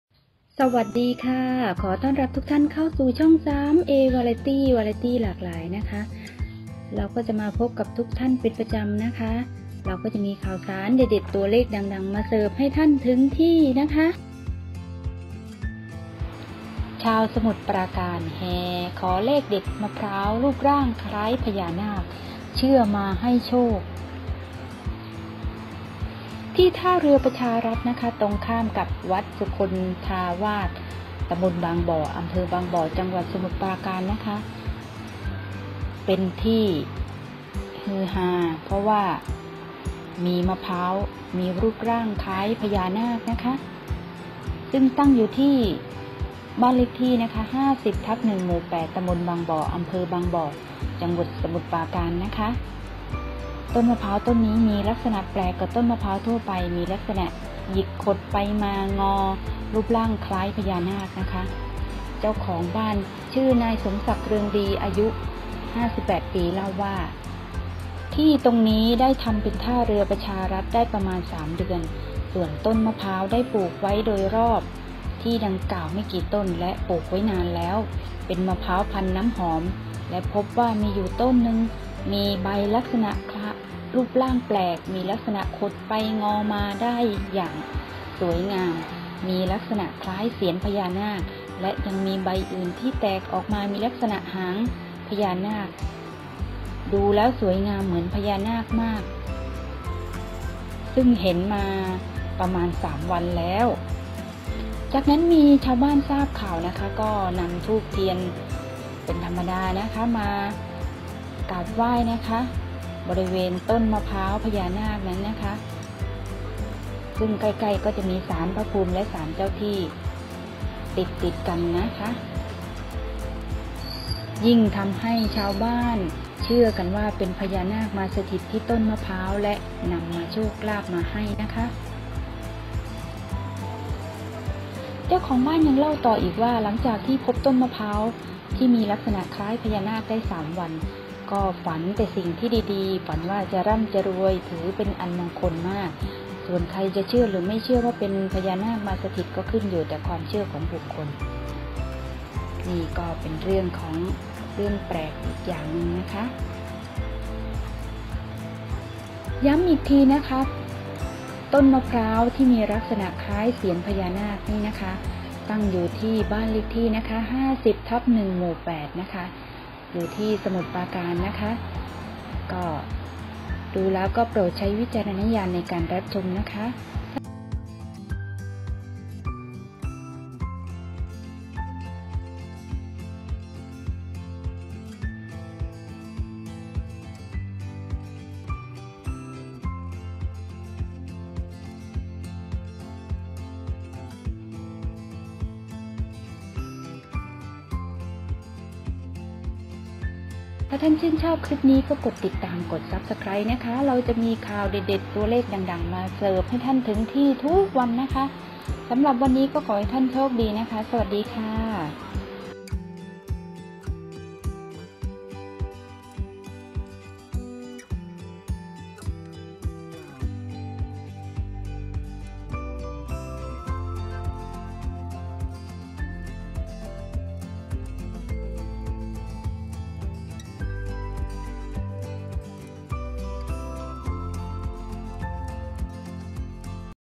ฮือฮา!มะพร้าวคล้าย"พญานาค"เจ้าของเชื่อมาให้โชค
ฮือฮา!มะพร้าวคล้าย"พญานาค"แปลกแต่จริง
*อยากได้เลขเด็ด หวยฟรี วาไรตี้หลากหลาย กดติดตาม หรือ กด subscribe และกดกระดิ่ง แจ้งเตือน ไว้เลยจ้า หรือกดตรงลิ้งข้างล่างนี้เลยค่ะ